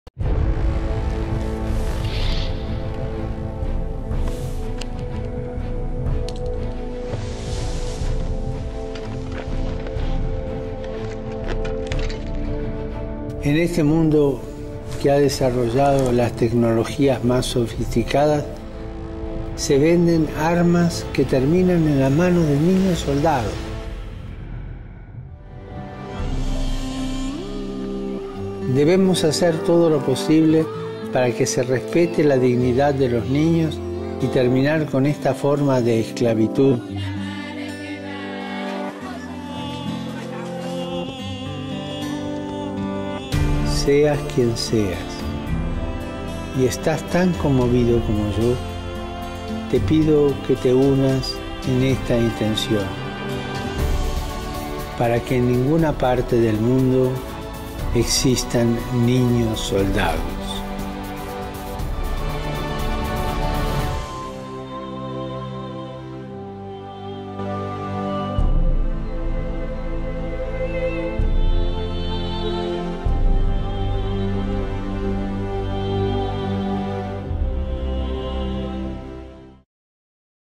P08 / 04/12/16 / 8/8b - El video del Papa: "Niños soldado"
EL VIDEO DEL PAPA.
Para que en ninguna parte del mundo existan niños soldados.
Papa Francisco - Diciembre 2016